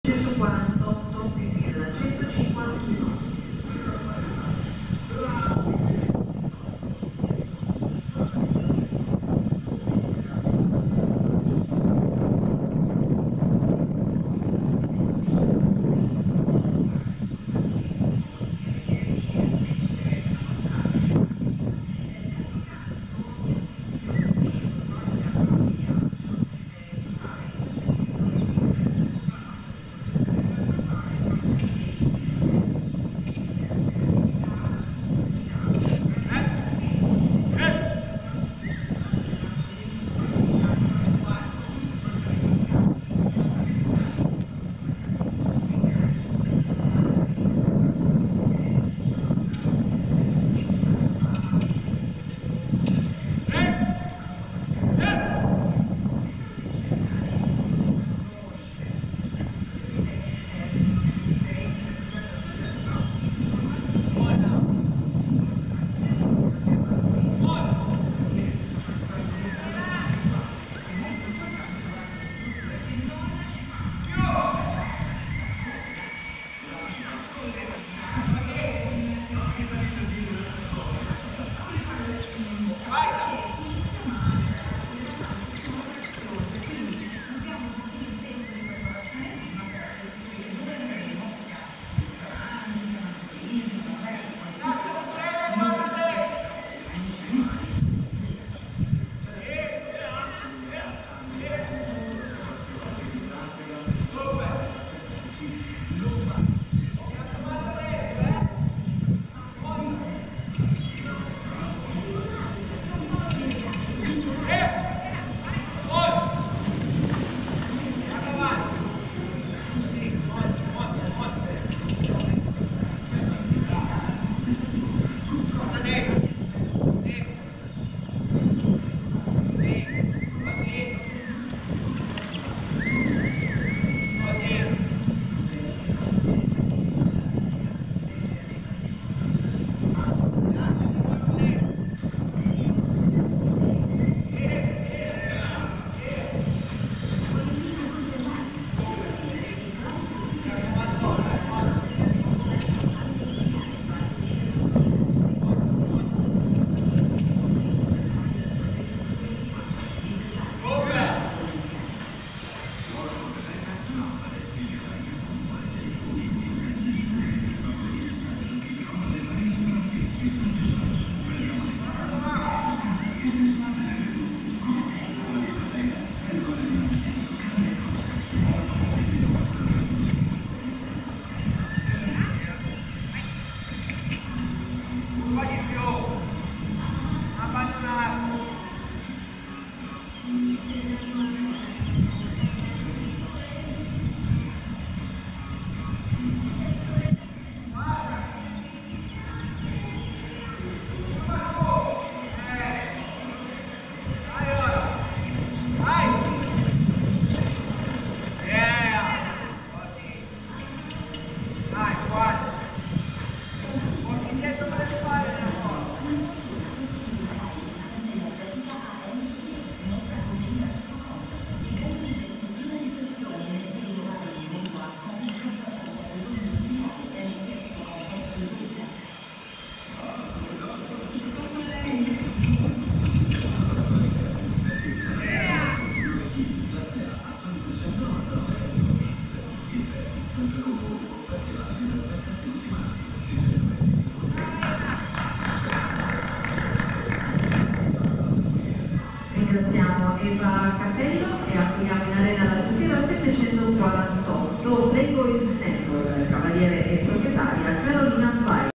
1 tappa APCR 2024 - CAPELLO EVA & RS WI-FI JAC score 71,5
CAPELLO EVA & RS WI-FI JAC owner CAPELLO MATTEO score 71,5 - APCR-IRHA-FISE YOUTH 14-18 & NRHA YOUTH 14-18 Champion - NRHA APCR WAITING FOR SPRING AT 23QH e 1 tappa APCR-IRHA-FISE-NRHA 2024 - credits Bonaga Communication